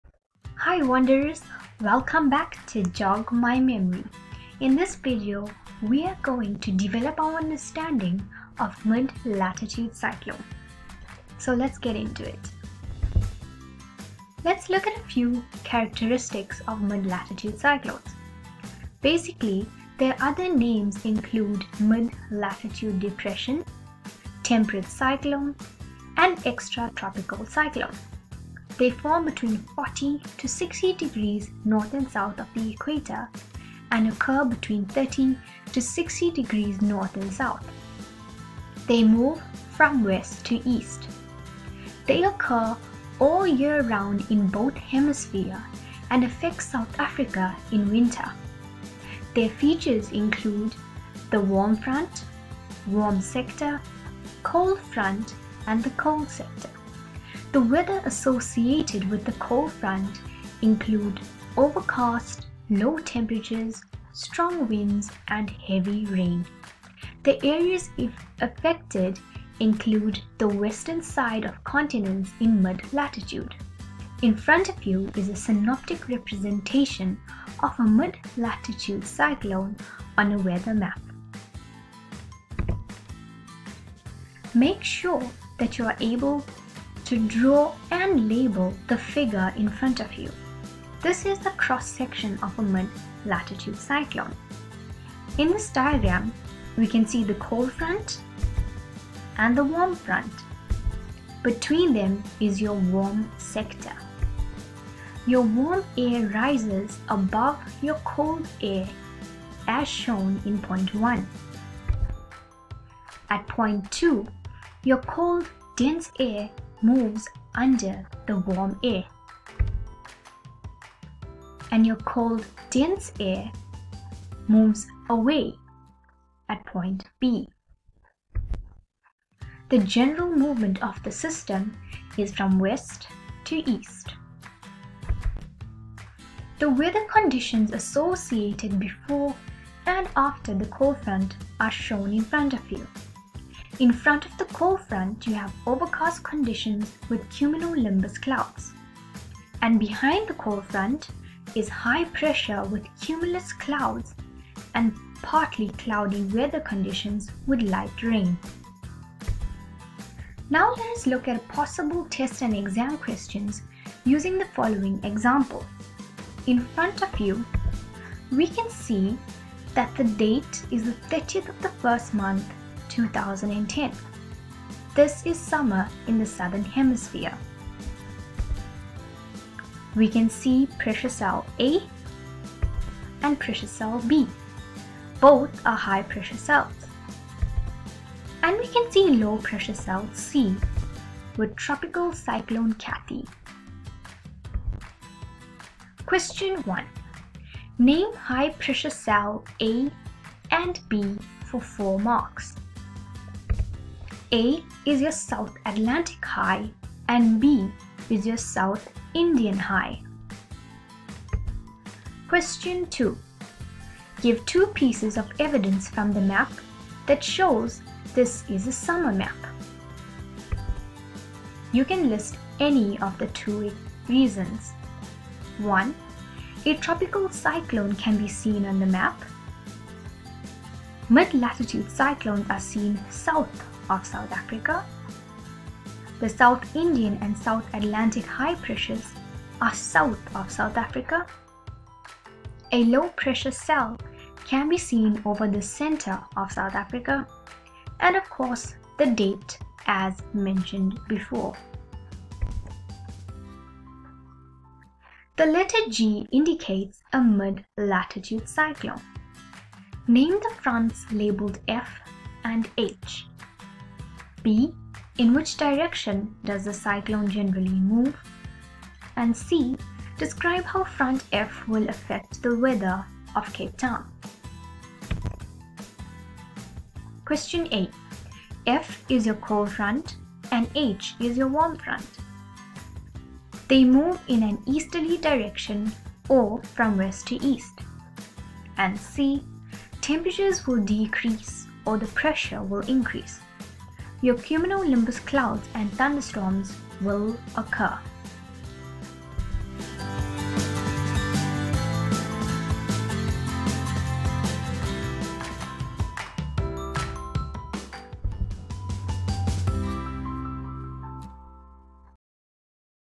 Grade 10-12 Geography: Climate and Weather- Mid-Latitude Cyclones
Improve your understanding of Mid-Latitude cyclones with possible exam questions and answers.